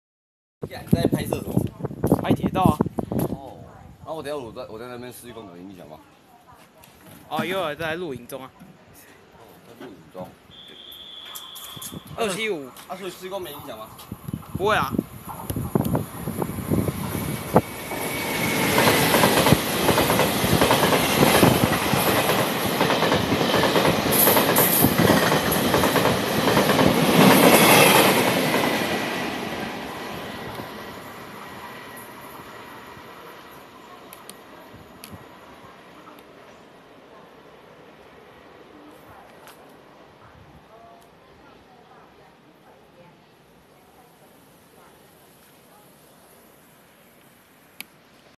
275次豬王自強號埔心站飆速通過，跟員工打斷我說話
275 經由  山線  花蓮→田中
備註:逢週日行駛
(田中端)E1055+1042(花蓮端)
12節親子車廂PPP2511
本次列車沿途停靠
花蓮，羅東，宜蘭，七堵，松山
臺北，板橋，桃園，中壢，新竹
竹南，苗栗，豐原，臺中，彰化
員林，田中。